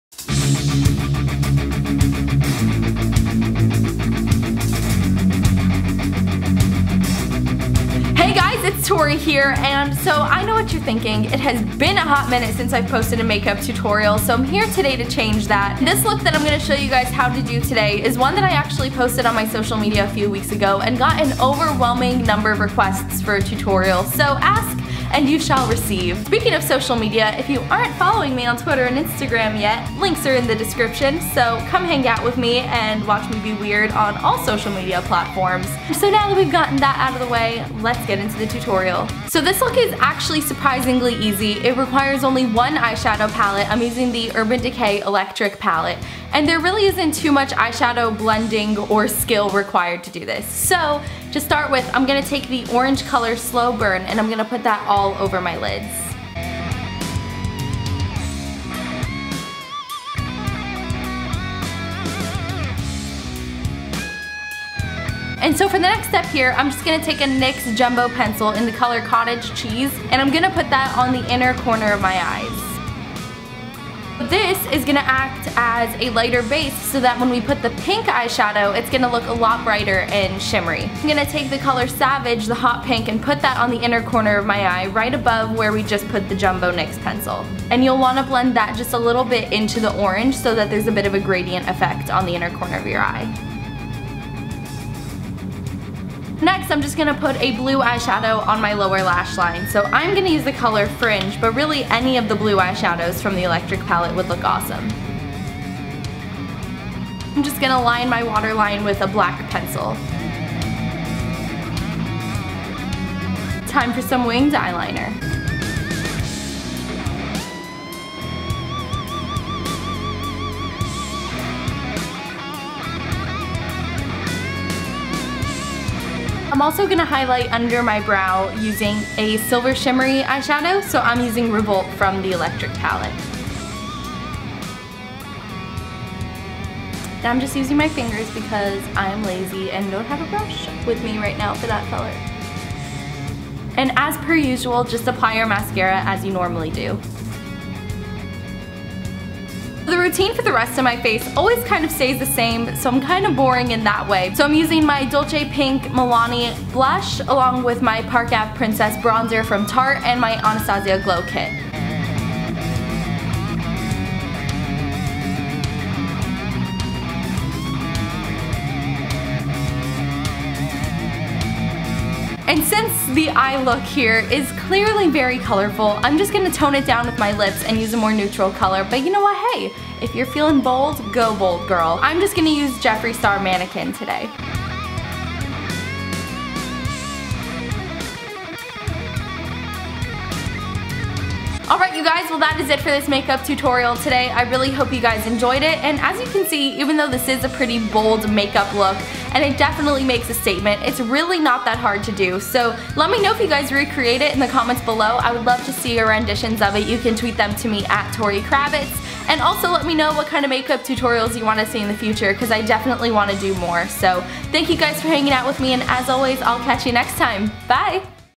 Easy Neon Eyes | Makeup Tutorial
Products used:
- Urban Decay Electric Palette (Slowburn, Savage, Fringe, Revolt)
- Kat Von D Tattoo Liner (Trooper)
- Urban Decay 24/7 Eye Pencil (Zero)
- Jeffree Star Liquid Lipstick (Mannequin)
- Tarte Park Ave Princess Contour Palette
- Anastasia Sun Dipped Glow Kit (Summer and Moonstone)
- Milani Baked Blush (Dolce Pink)